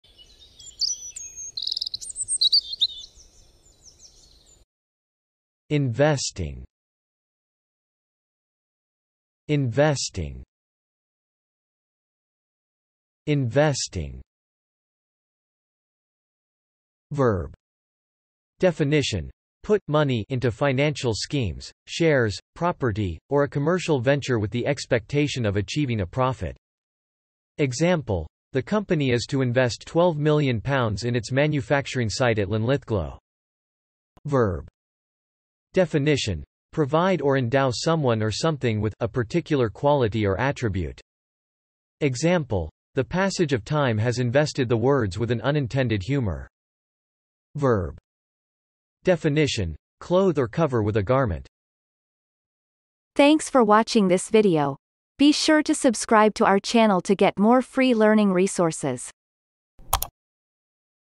How to pronounce investing - Vocab Today
The pronunciation of the word investing in Amercian accent is demonstrated in this video. If the word is from another language, such as brand name, it will be pronounced in its original language. Based on the word frequency, this is of 2 stars difficulty.

The word investing has the following meanings:
1. (verb)
Definition : Put (money) into financial schemes, shares, property, or a commercial venture with the expectation of achieving a profit.
Example : the company is to invest £12 m in its manufacturing site at Linlithglow
2. (verb)
Definition : Provide or endow someone or something with (a particular quality or attribute)
Example : the passage of time has invested the words with an unintended humour
3. (verb)
Definition : Clothe or cover with a garment.